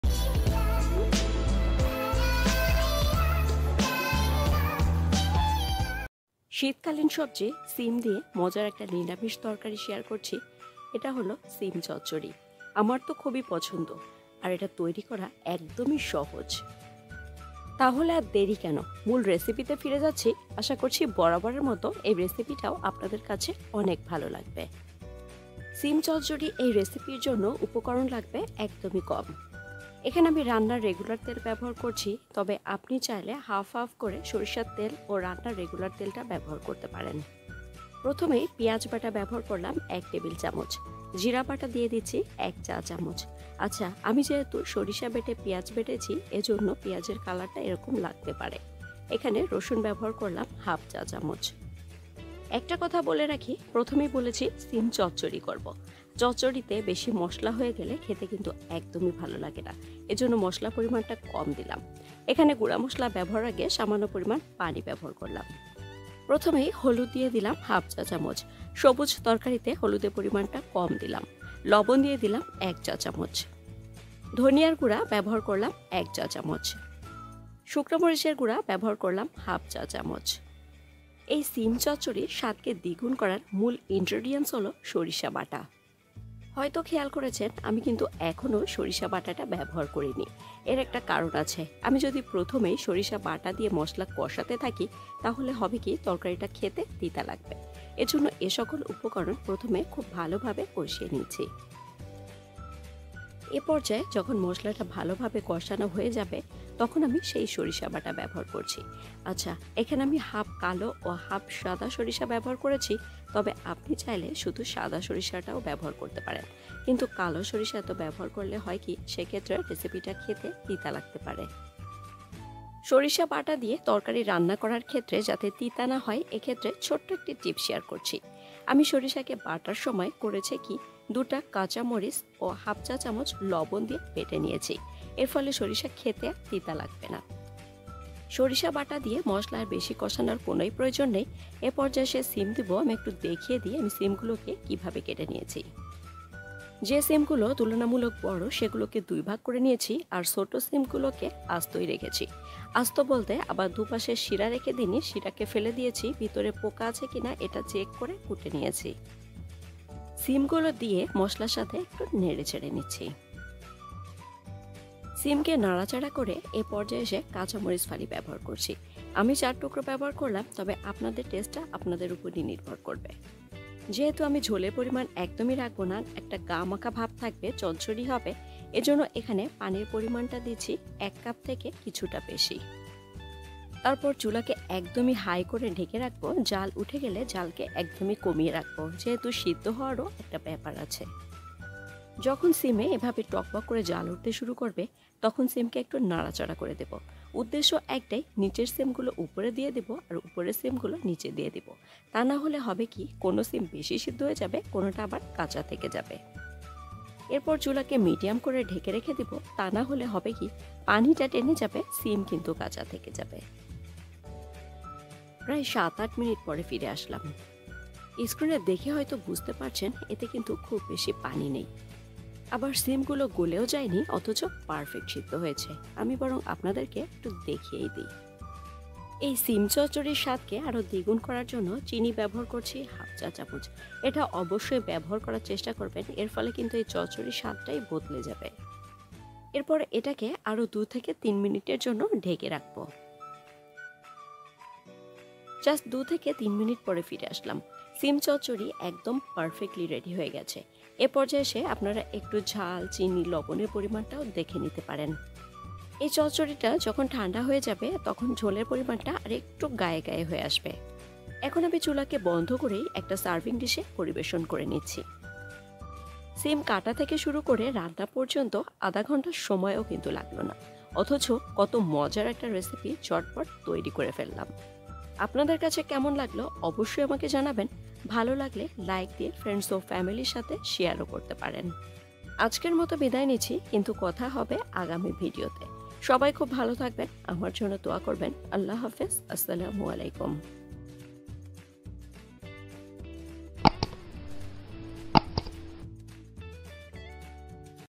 শিমের এই রেসিপিটি খেলে মন ভরে যাবে | শিম চচ্চড়ি | সর্ষে শিম | Sim Chochori
সর্ষে বাটায় শিম চচ্চড়ি খেলে আসলেই মন ভরে যায়। আর এটি তৈরি করা খুব সহজ।